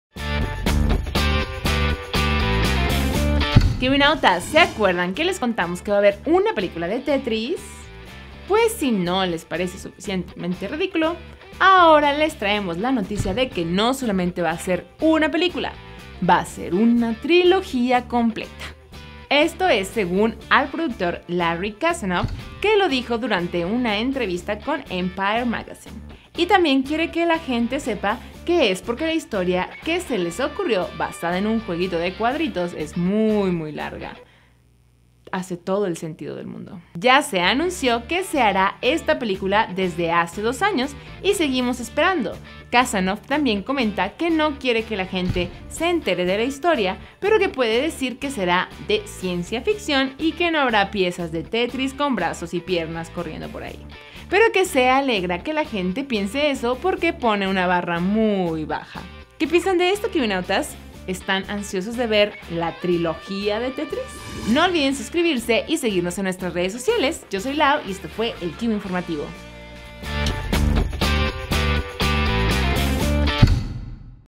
Película de Tetris será trilogía
La historia de un juego de cuadritos va a ser muy muy larga.

Créditos:

Conducción:
Laura Figón @laury992

Escrito por:
Oscar Perez @pwrofpwr

Postproducción:
Jezabel VM @jzbl14